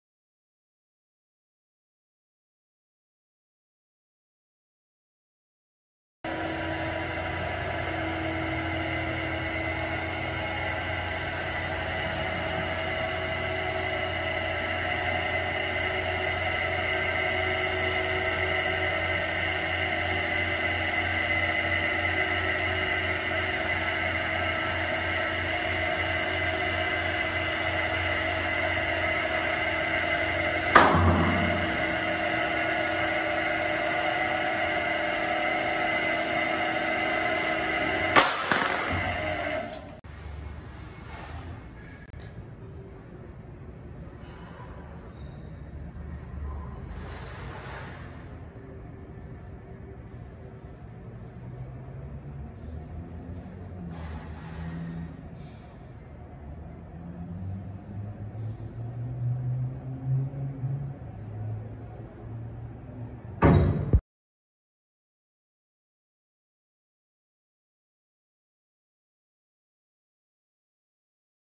Global Industrial Electric Powered Hydraulic Lift Box HBD436 & Container Dumper, CCR15897
Brand: Global Industrial
Serial: S182286
Model: HBD436
Dump Capacity: 4000 Lbs.
Motor: AllWIN Motors
Model: YG88-4
HP: 2
RPM: 1440 / 1725
Hz: 50 / 60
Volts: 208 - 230
Volts: 460
Phase: 3
Usable Lift Dimensions: 52 In. x 50 In.
Overall Dimensions: 69 In. L x 70 In. W x 59.5 In. H
Dumper Is Fully Functional But Will Need To Be Wired In.

CCR15897